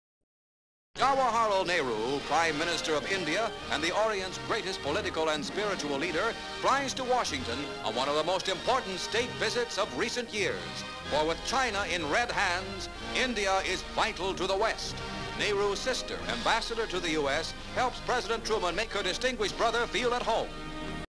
Nehru In Usa (1949)
American newsreel item.

Indian Prime Minister Pandit Nehru in Washington DC, United States of America (USA).

Various shots Nehru alighting from aeroplane.  Nehru is greeted by President Harry Truman.   Elevated shots Truman and Nehru walking across airfield.  CU Nehru sitting in car, pan to Mrs. Pandit (Nehru's sister), Indian Ambassador to the US.  CU Nehru, Mrs Pandit and President Truman seated in. car.

 FILM ID:2521.19